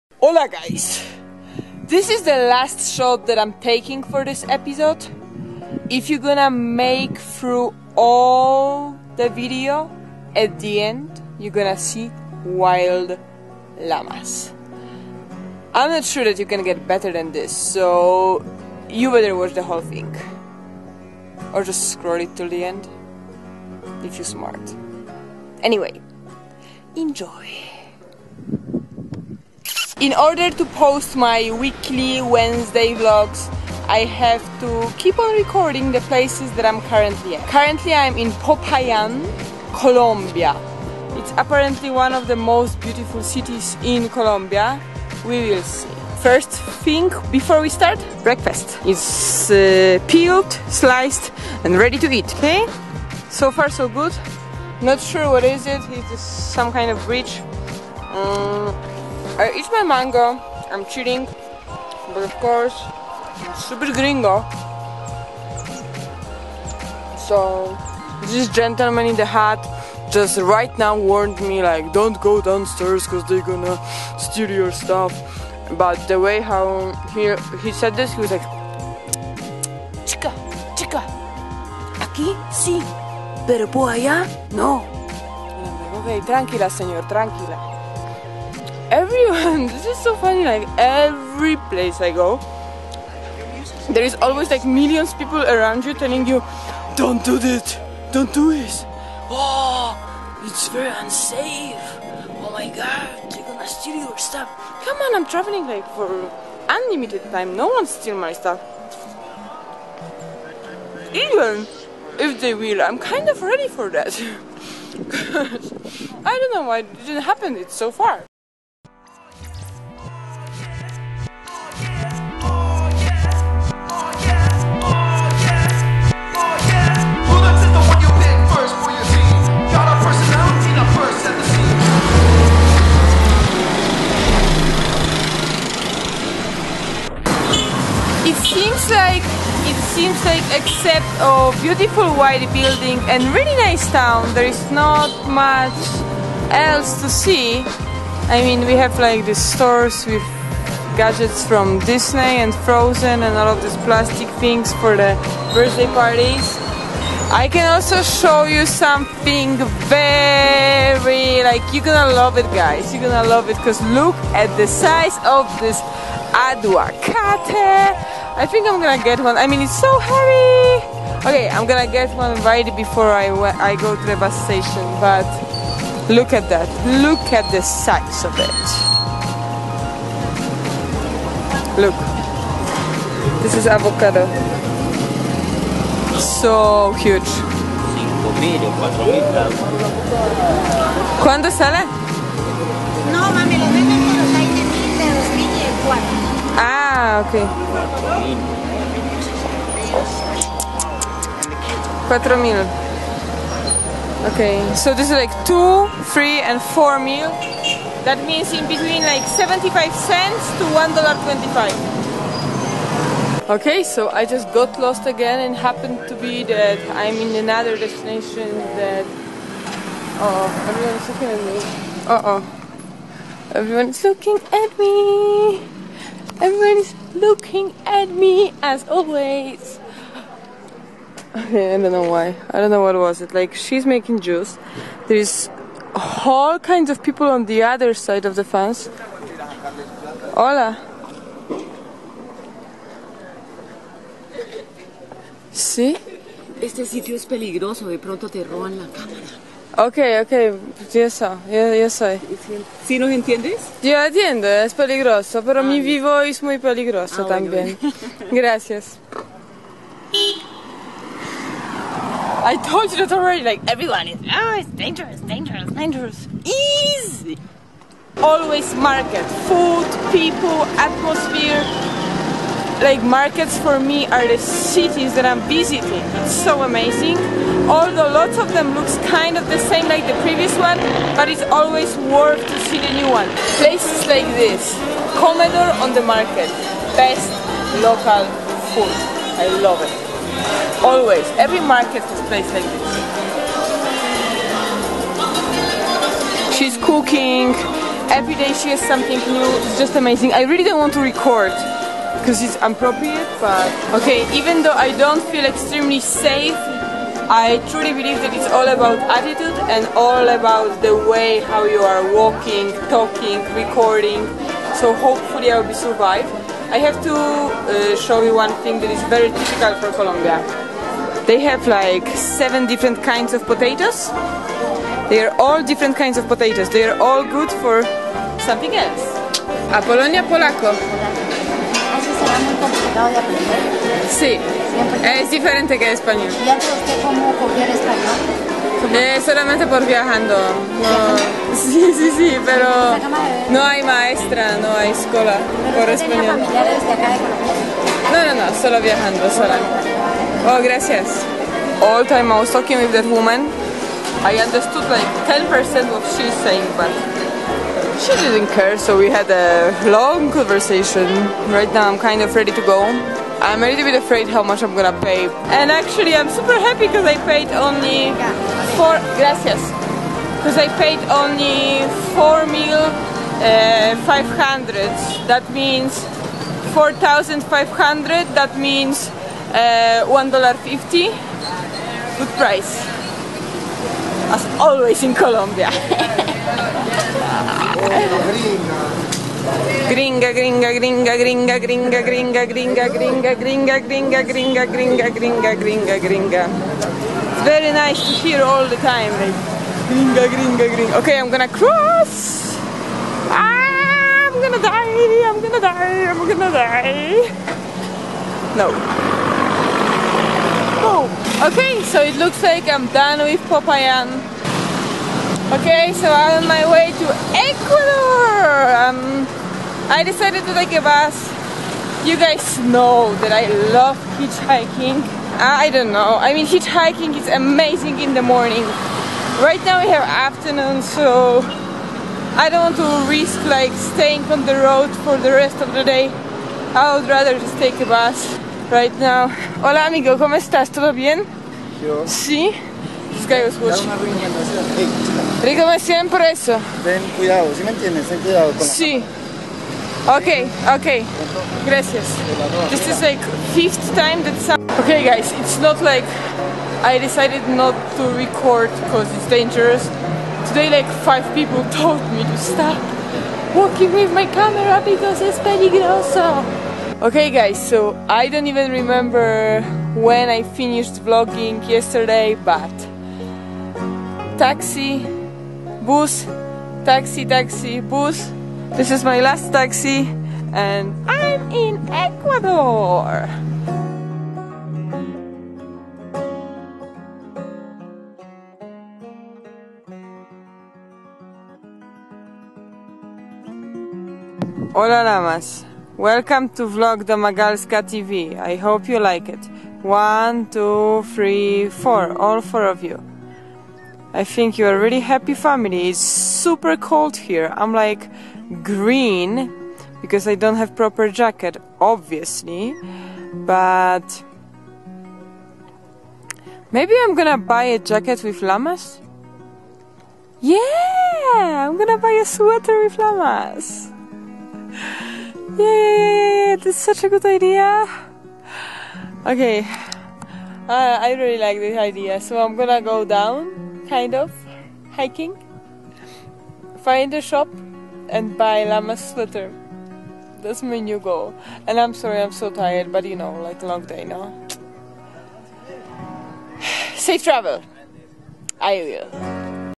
"successful"DAILY VLOG in POPAYAN (feat. lamas in Ecuador) / vlog #74
I'm visiting Popayan, beautiful city in south Colombia and really trying to record while local people are keep warning me how dangerous it is and that apparently "some people" wanted to still my camera. Fuck that people just let me record MY THING. I believe in Positive energy and Good vibes, no one will attract me. Later that day I'm crossing border with Ecuador so as bonus You can see a little bit of panorama in Quito, Ecuador. KEEP POSITIVE GUYS! STAY STRONG!